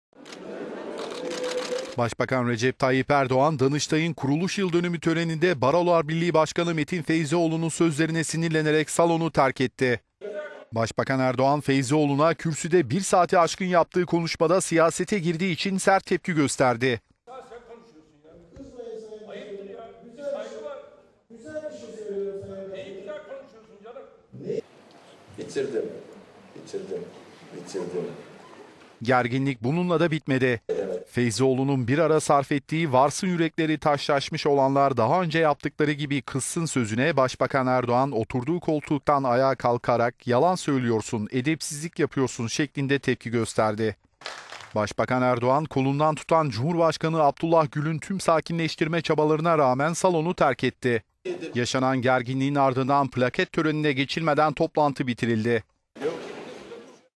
Başbakan Erdoğan, Feyzioğlu'na sinirlenerek töreni terk etti
Başbakan Recep Tayyip Erdoğan, Danıştay'ın kuruluş yıldönümü töreninde Barolar Birliği Başkanı Metin Feyzioğlu'nun sözlerine sinirlenerek salonu terk etti.

Başbakan Recep Tayyip Erdoğan, Metin Feyzioğlu'nun yaptığı konuşmada siyasete girdiği gerekçesiyle tepki gösterdi.

Feyzioğlu'nun bir ara sarf ettiği "Varsın yürekleri taşlaşmış olanlar daha önce yaptıkları gibi kızsın" sözü üzerine Başbakan Erdoğan oturduğu koltuktan ayağa kalkarak "yalan konuşuyorsun, edepsizlik yapıyorsun, Van'da neler ya...